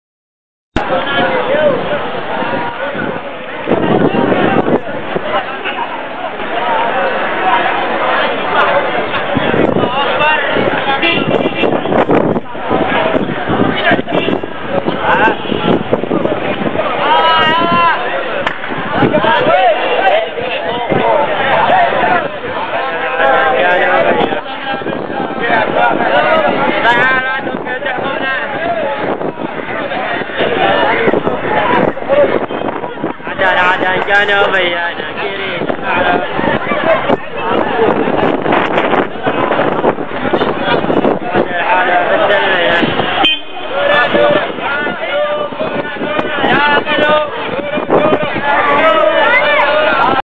اللجان الشعبية الجنوبية تسيطر على مبنى الأمن العام خور مكسر عدن وترفع العلم الجنوبي
خور مكسر العاصمة عدن